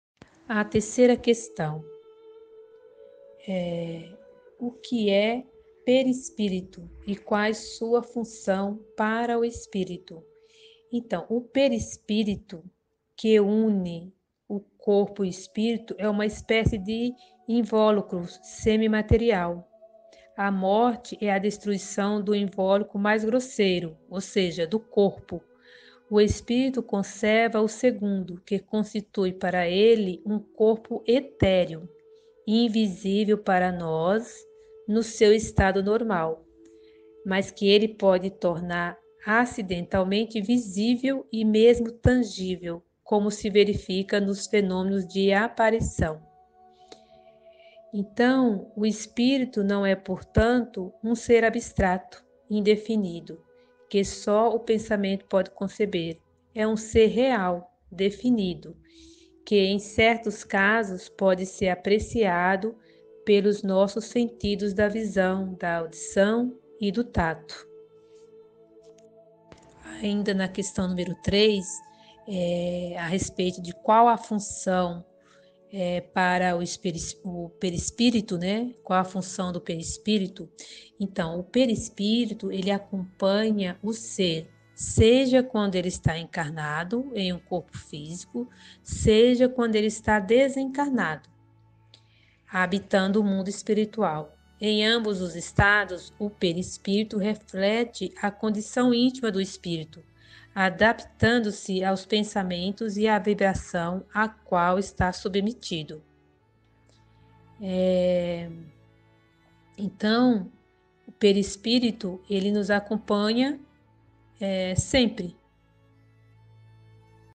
O que é períspirito? Qual é a sua função para o espírito?
Continuação da introdução do livro dos espíritos.